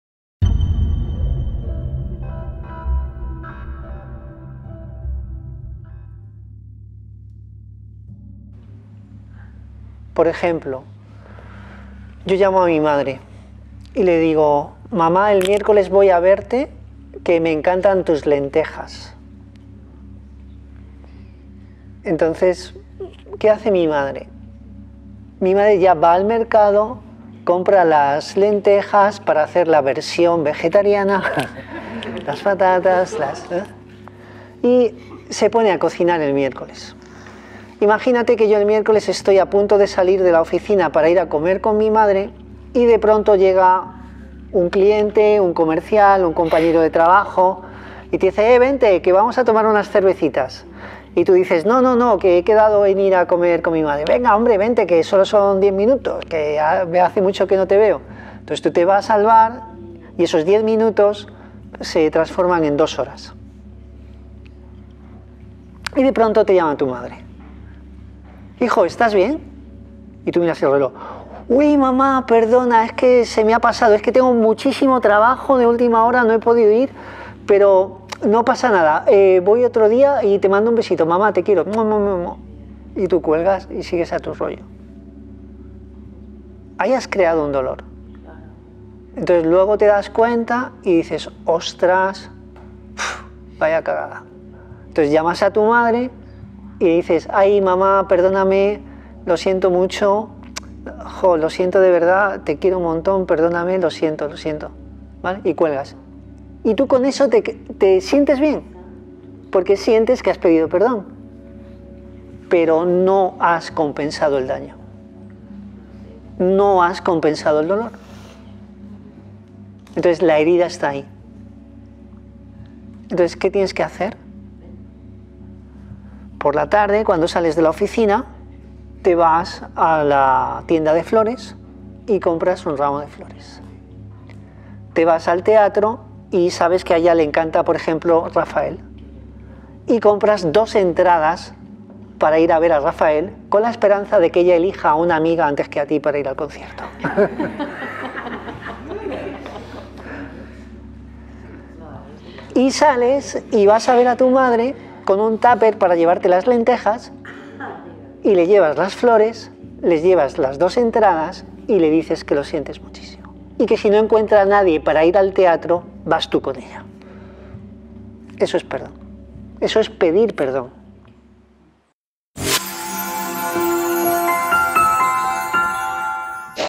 Pedir perdón 🙏​
Hola, queridos seguidores!
🟢 𝗔𝗽𝘂́𝗻𝘁𝗮𝘁𝗲 𝗚𝗥𝗔𝗧𝗜𝗦 𝗮𝗹 𝗿𝗲𝘁𝗼 "𝗘𝗹 𝗣𝗼𝗱𝗲𝗿 𝗱𝗲𝗹 𝗦𝗼𝗻𝗶𝗱𝗼"⁣
9 días → 9 frecuencias de sonido muy poderosas que generarán en ti cambios muy potentes.

Pedir perdón es muy importante para estar bien con uno mismo y para compensar el daño generado a las personas afectadas.
En esta ocasión quiero compartir con vosotros un tema muy personal: la importancia de pedir perdón y de cómo pedirlo.
Por mucho que nos duela, muchas veces nos empezaremos a sentir mejor una vez que hemos hecho la acción asociada a pedir perdón. Así nos sentiremos mejor con nosotros mismos, nuestros familiares y nuestros amigos.